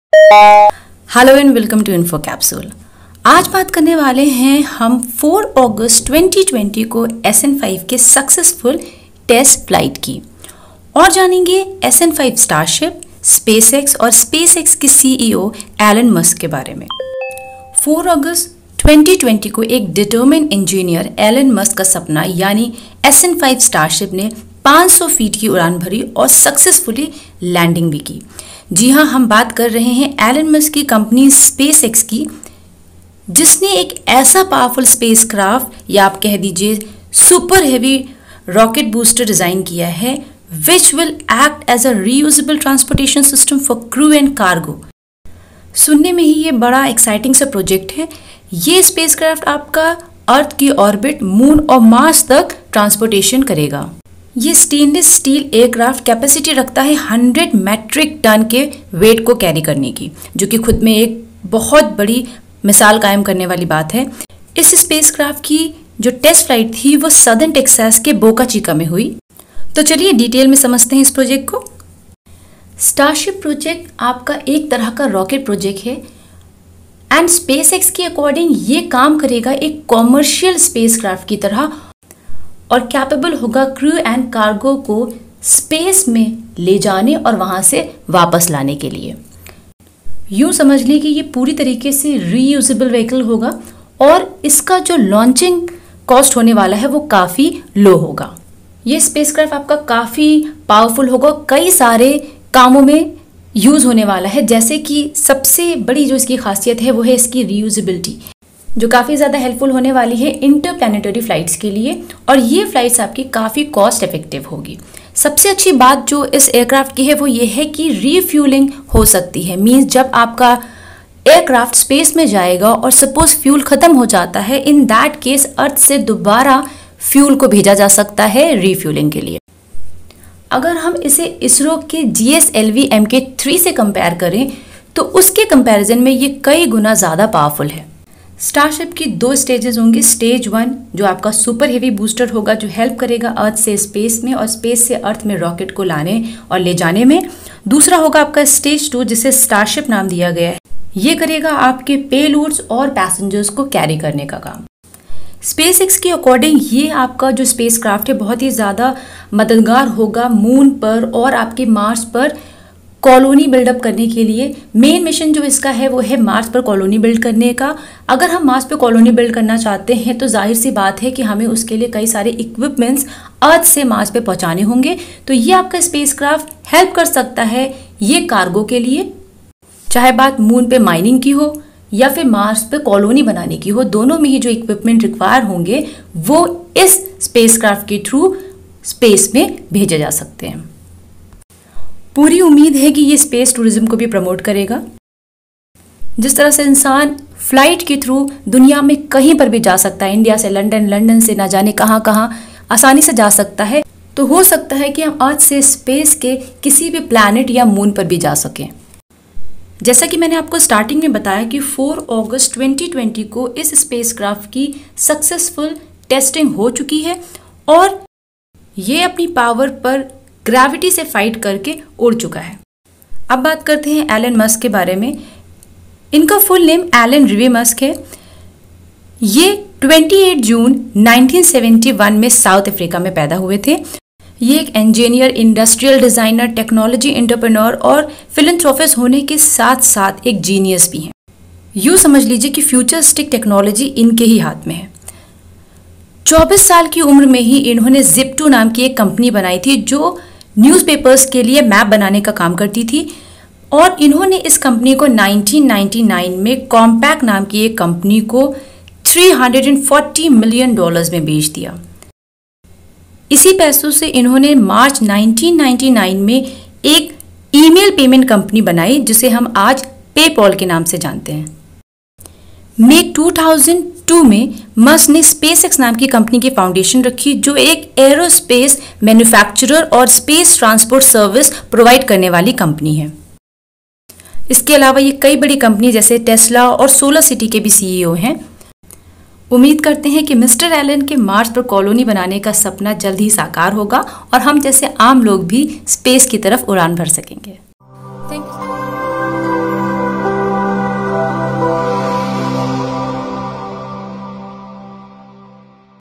Mars Starship prototype: Everything you need to know मंगल स्टारशिप प्रोटोटाइपःसंपूर्ण जानकारी
This video gives an insight about the Elon Musk's Starship designed by his company SpaceX. It is a spacecraft and super-heavy booster rocket meant to act as a reusable transportation system for crew and cargo to the Earth’s orbit, Moon and Mars.

यह वीडियो एलन मस्क की कंपनी स्पेस-एक्स द्वारा डिज़ाइन की गयी स्टारशिप के बारे में जानकारी देता है। स्टारशिप एक अंतरिक्ष यान और सुपर-भारी बूस्टर रॉकेट है जो कि पृथ्वी की कक्षा, चंद्रमा और मंगल पर चालक दल और कार्गो के लिए पुन: प्रयोज्य परिवहन प्रणाली के रूप में कार्य करेगा।